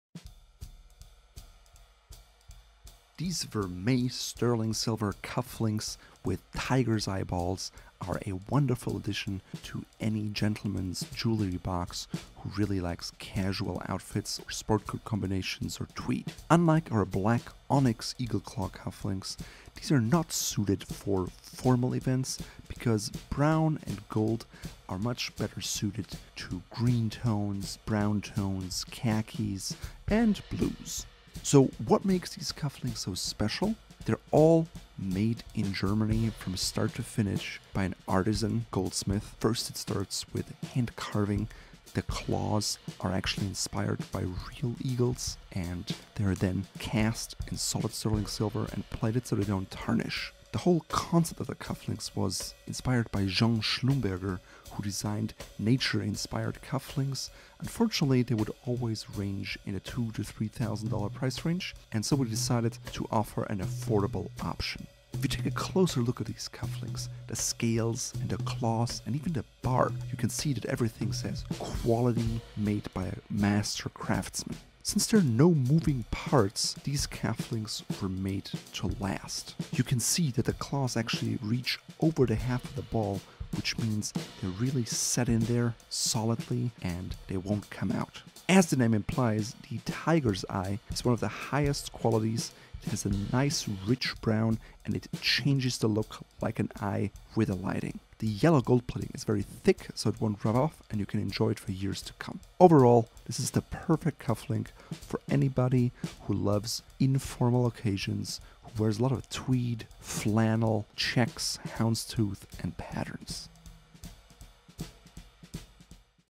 Tiger's Eye Cufflinks with Eagle Claws - 925 Sterling Silver Gold Plated
These vermeil sterling silver cufflinks with tiger's eyeballs are a wonderful addition to any gentleman's jewelry box who really like casual outfits or sport coat combinations or tweed.

Unlike our black onyx eagle claw cufflinks, these are not suited for formal events because brown and gold are much better suited to green tones, brown tones, khakis, and blue tones.

So what makes these cufflinks so special? They are all made in Germany from start to finish by an artisan goldsmith. First starts with hand carving, the claws are actually inspired by real eagles and they are then cast in solid sterling silver and plated so they don't tarnish.

The whole concept of our cufflinks was inspired by Jean Schlumberger who designed nature inspired cufflinks. Unfortunately, they would always range in a $2000 price range and so we decided to offer an affordable option.

If you take a closer look at these cufflinks, the scales, the claws. and even the bark, you can see that everything says quality made by a master craftsman.

Since there are no moving parts, these cufflinks were made to last. You can see that the claws actually reach over the half of the ball which means they're really set in there solidly and they won't come out.

As the name implies, the tiger's eye is one of the highest qualities, has a nice rich brown and it changes the look like an eye with the lighting.

The yellow gold plate is very thick so it won't come off and you can enjoy it for years to come.

Overall, this is the perfect cufflink for anybody who loves informal occasions, who has a lot of tweed, flannel, checks, houndstooth, and patterns.